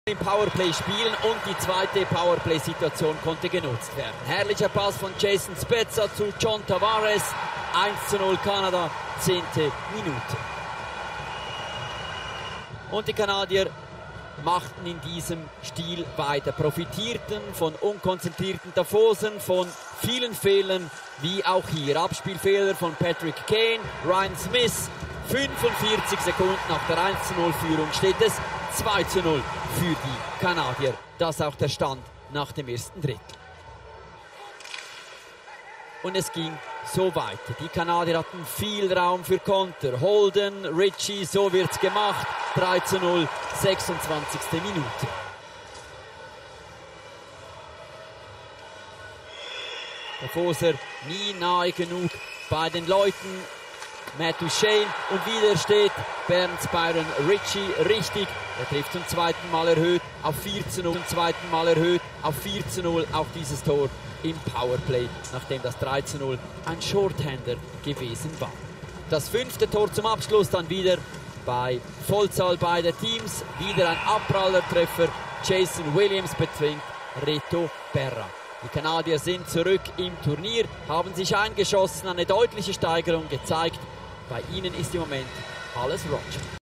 Spengler Cup 2012 : HC Davos vs. Team Canada (0:5)
Titelverteidiger HC Davos hat am 86. Spengler Cup eine bittere Startniederlage einstecken müssen. Das Team von Arno Del Curto unterlag in der Gruppe Cattini nach einer fehlerhaften Leistung dem Team Canada diskussionslos mit 0:5.